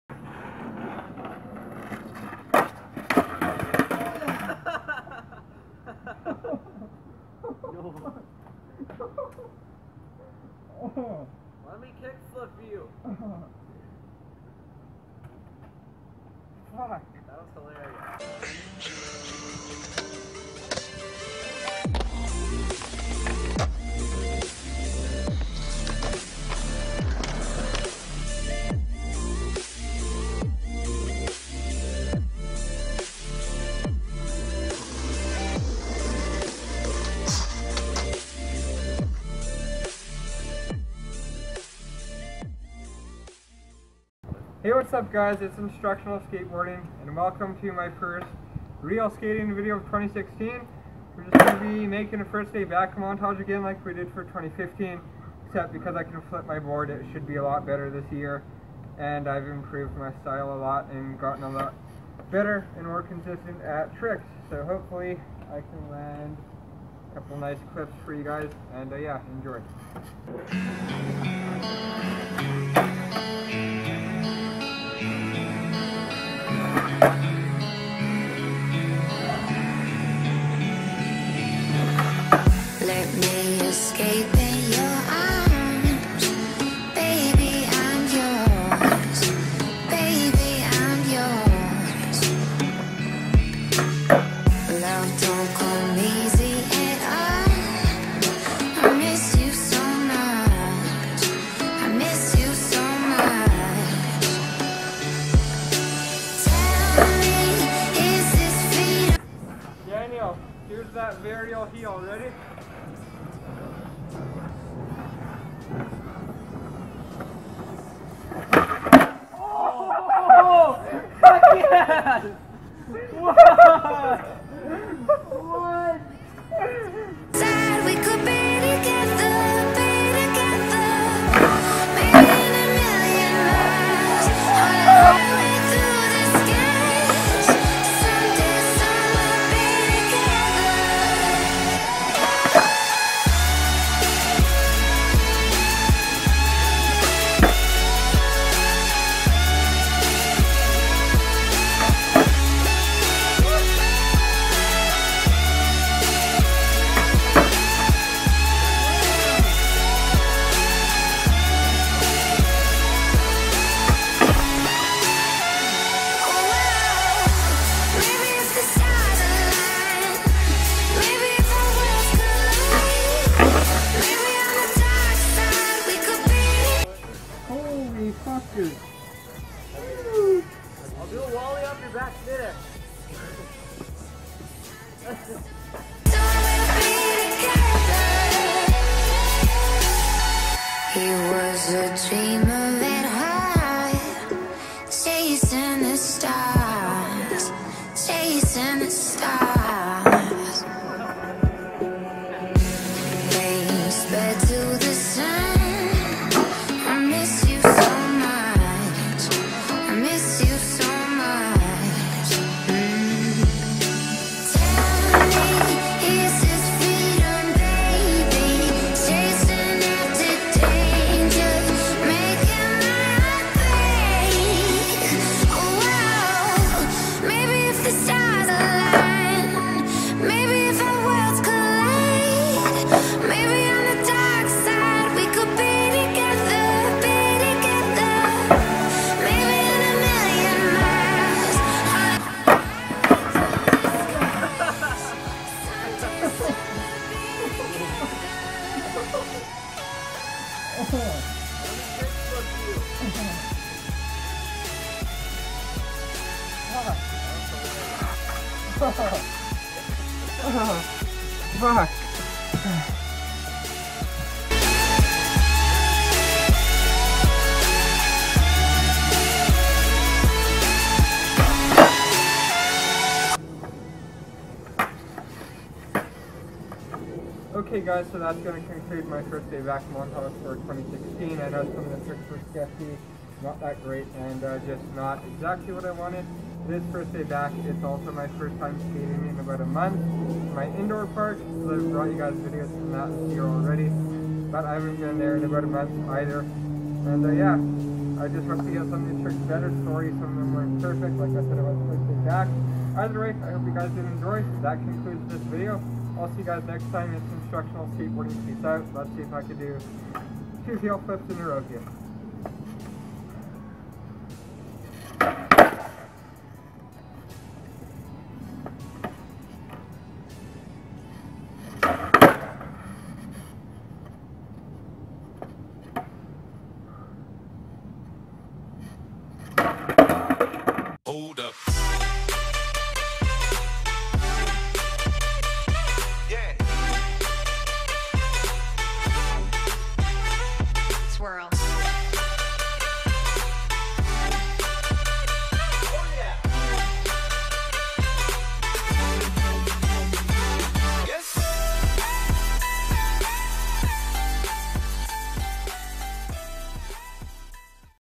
"FIRST DAY BACK" SKATEBOARDING MONTAGE 2016
Although I have somewhat been skating throughout the winter at my local park, this is my first REAL day back and back in my comfort zone. Hope you guys enjoy these clips. I'm no god, but couldn't wait to get back to youtube so here ya go :)

Previous video: 5 STAIR POP SHUVIT REDEMPTION W BONUS FOOTAGE